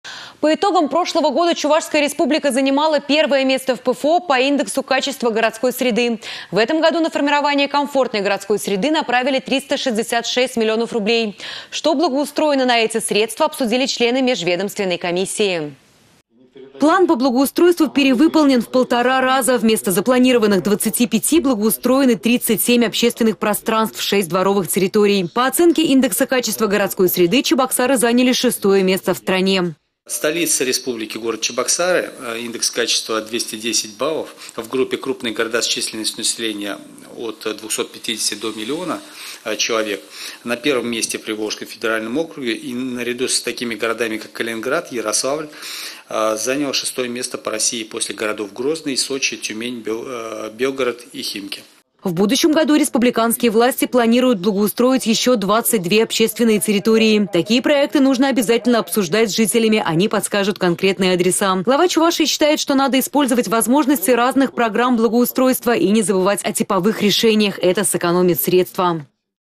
Итоги формирования комфортной городской среды
По итогам прошлого года Чувашская Республика занимала первое место в ПФО по индексу качества городской среды. В этом году на формирование комфортной городской среды направили 366 млн рублей. Что благоустроено на эти средства - обсудили члены Межведомственной комиссии.